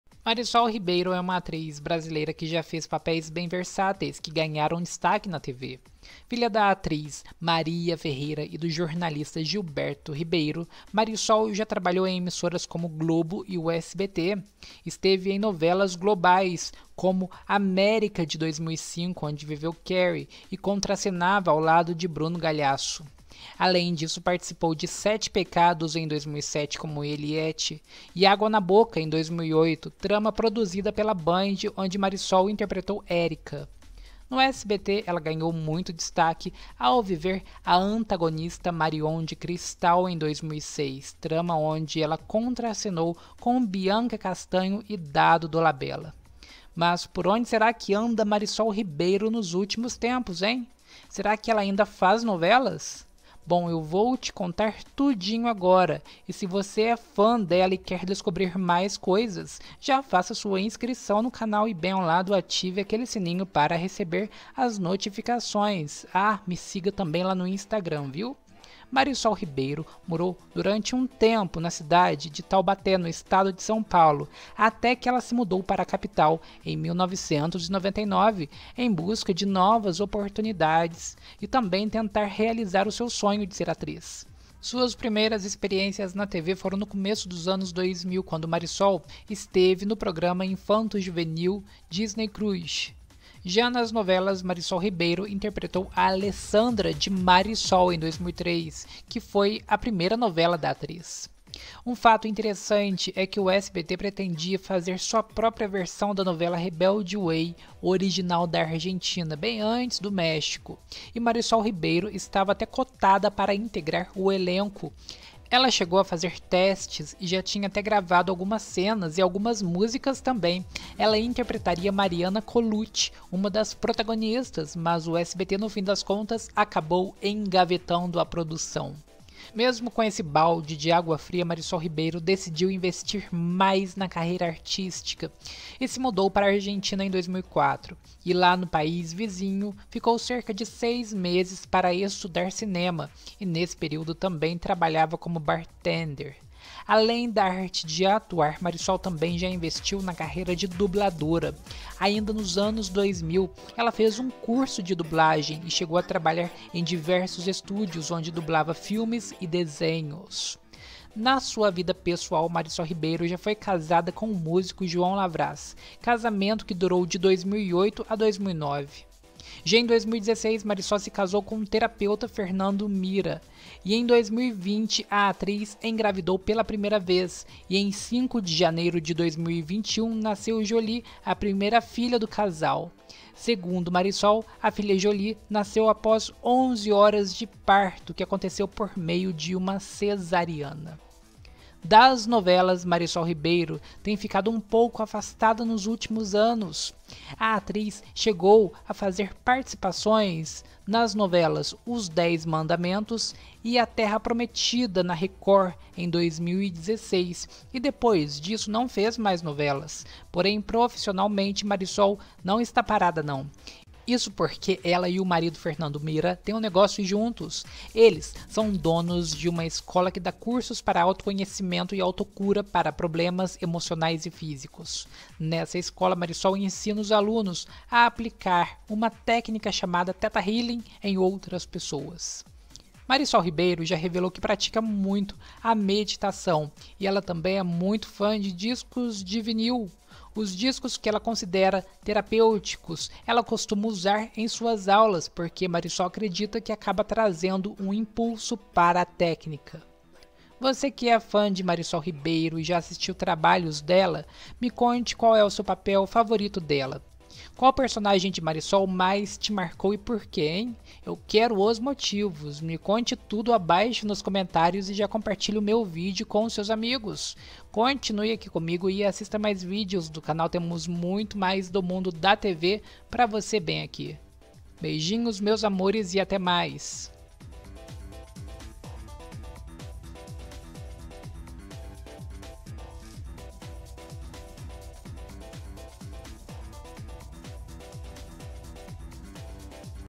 Por onde anda MARISOL RIBEIRO? ela interpretou a vilã MARION em
Saiba mais da atriz Marisol Ribeiro que já participou das novelas "América", "Sete Pecados" e "Morde & Assopra". Ah, me siga tbm lá no insta @diogototalon